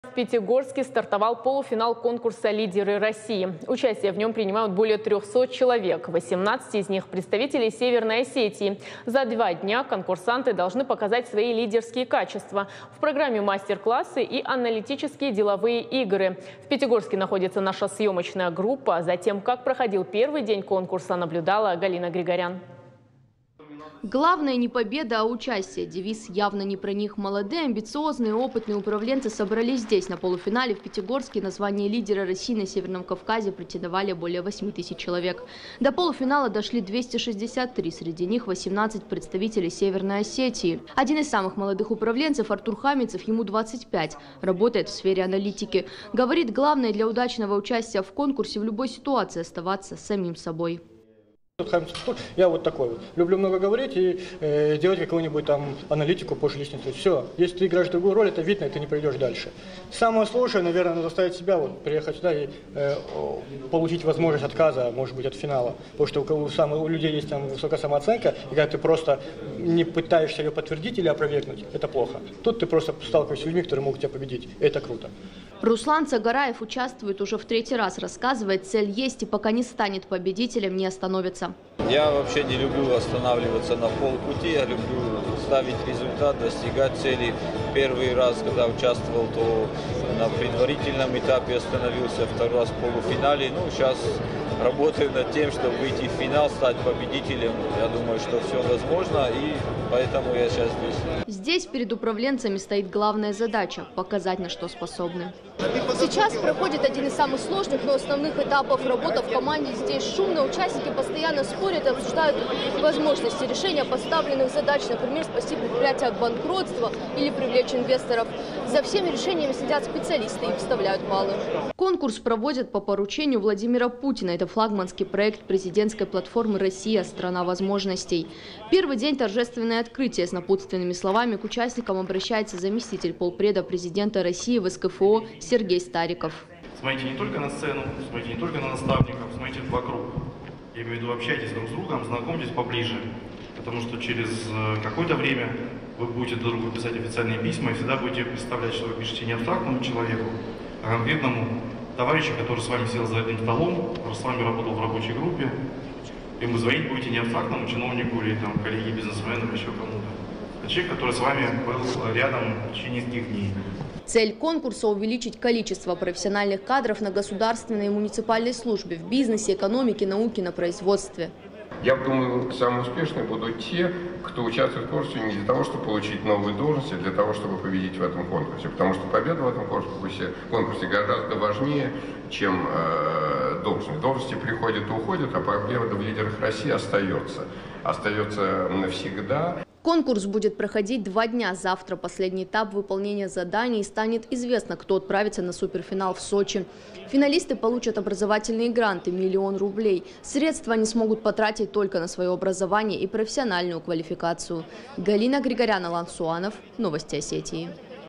В Пятигорске стартовал полуфинал конкурса «Лидеры России»
В Пятигорске стартовал полуфинал конкурса «Лидеры России». Участие в нем принимают более 300 человек. 18 из них - представители Северной Осетии. За два дня конкурсанты должны показать свои лидерские качества. В программе мастер-классы и аналитические деловые игры.

Главное не победа, а участие - девиз явно не про них. Молодые, амбициозные, опытные управленцы собрались здесь, на полуфинале в Пятигорске. На звание лидера России на Северном Кавказе претендовали более 8 тысяч человек, до полуфинала дошли 263. Среди них 18 представителей Северной Осетии. Один из самых Молодых управленцев Артур Хамицев, ему 25 работает в сфере аналитики. Говорит, главное для удачного участия в конкурсе в любой ситуации оставаться самим собой.

Сейчас проходит один из самых сложных, но основных этапов. Работа в команде. Здесь шумно, участники постоянно спорят и обсуждают возможности решения поставленных задач. Например, спасти предприятие от банкротства, привлечь инвесторов и многое другое. За решениями следят специалисты и выставляют баллы.

Конкурс проводят по поручению Владимира Путина. Это флагманский проект президентской платформы «Россия - страна возможностей». В первый день - торжественное открытие. С напутственными словами к участникам обращается заместитель полпреда президента России в СКФО - Сергей Стариков.

Цель конкурса увеличить количество профессиональных кадров на государственной и муниципальной службе, в бизнесе, экономике, науке, на производстве.

Конкурс будет проходить два дня. Завтра последний этап выполнения заданий, и станет известно, кто отправится на суперфинал в Сочи. Финалисты, получат образовательные гранты миллион рублей. Средства они смогут потратить только на свое образование и профессиональную квалификацию.
Мы рядом в:
ok.ru/irystontv